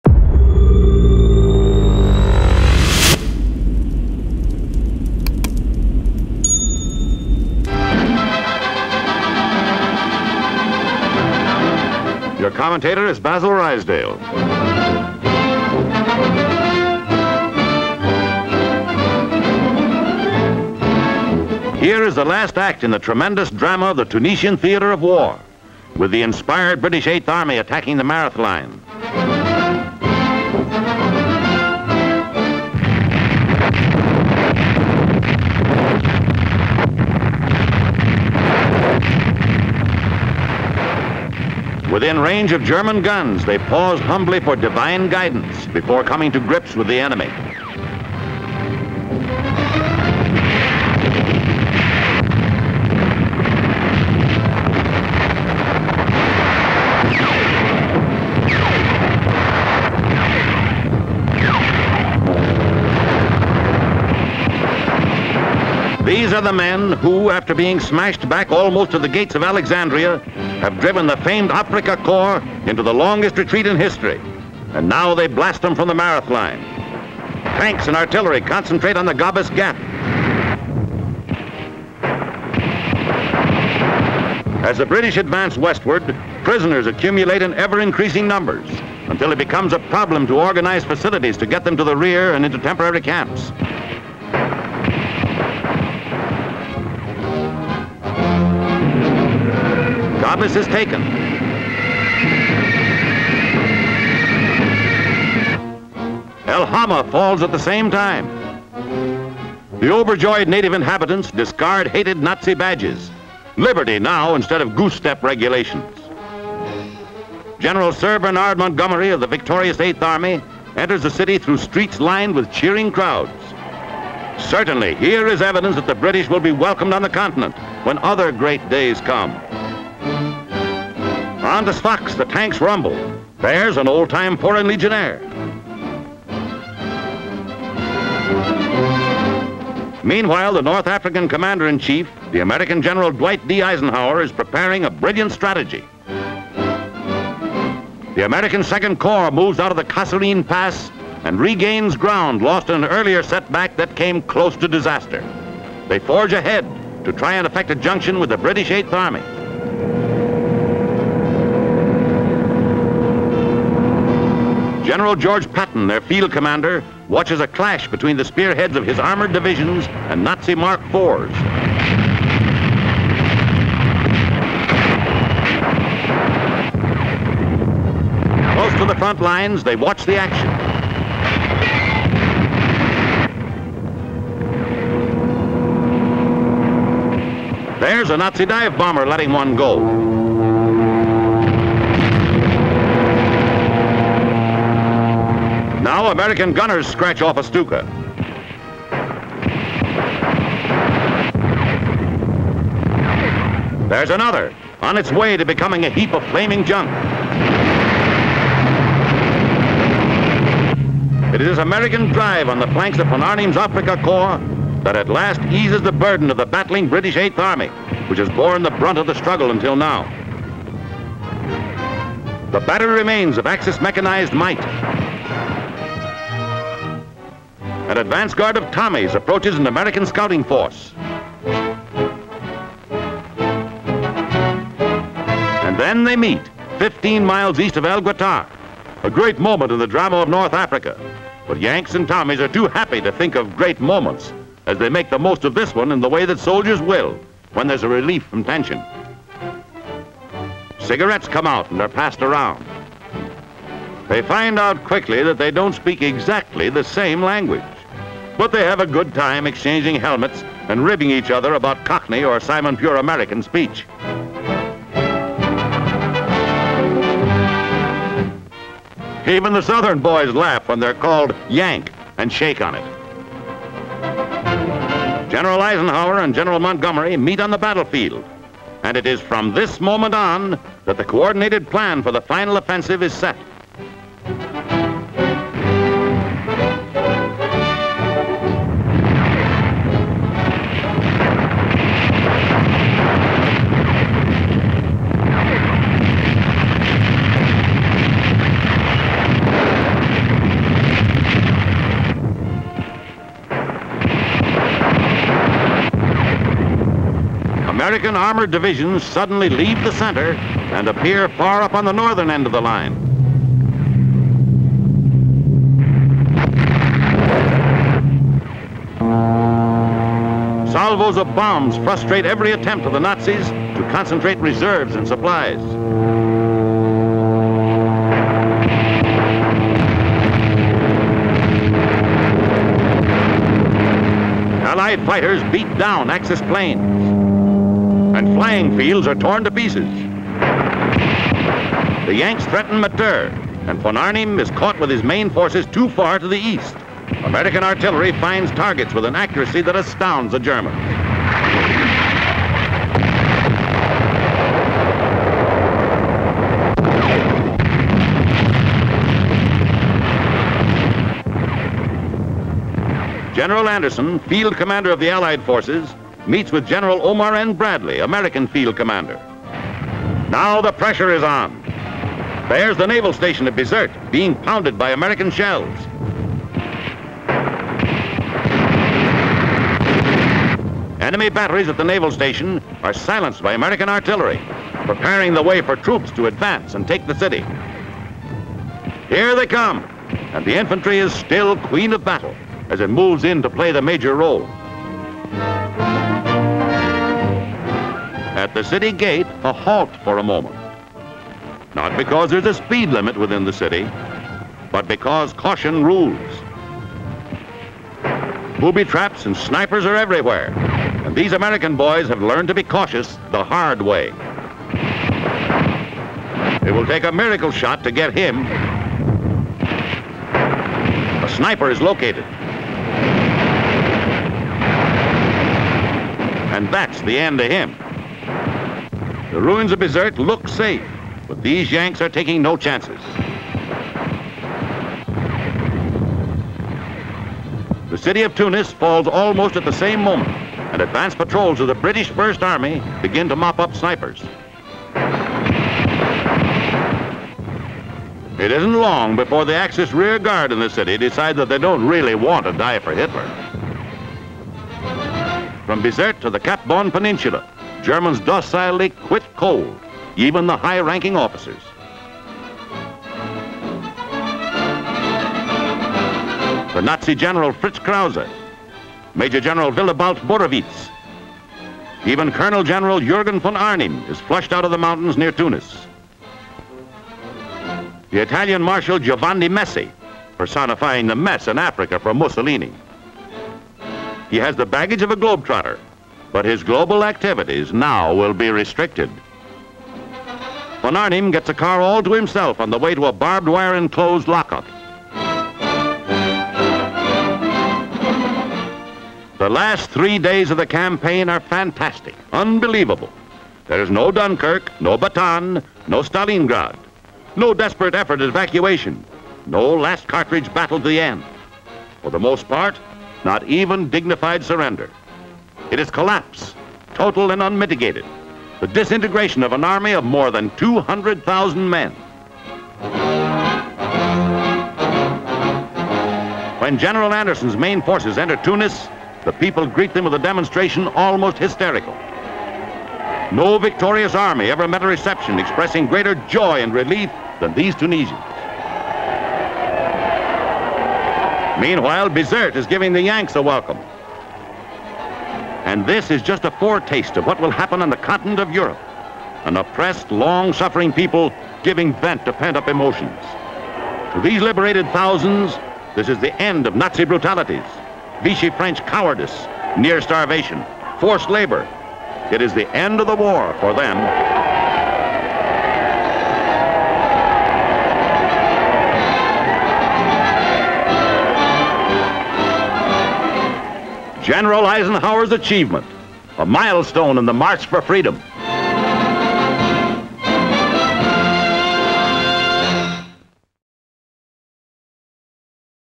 Axis smashed in North Africa | Eisenhower's Tunisian Campaign | HD - Audio restored
In 1943, after battles across North Africa against Nazis, Montgomery and Eisenhower’s armies meet up.
Tunisia is liberated and the celebration is seen as a forerunner of victories to come in Europe.

- Footage upscaled to 1080p.
- Audio cleaned and restored.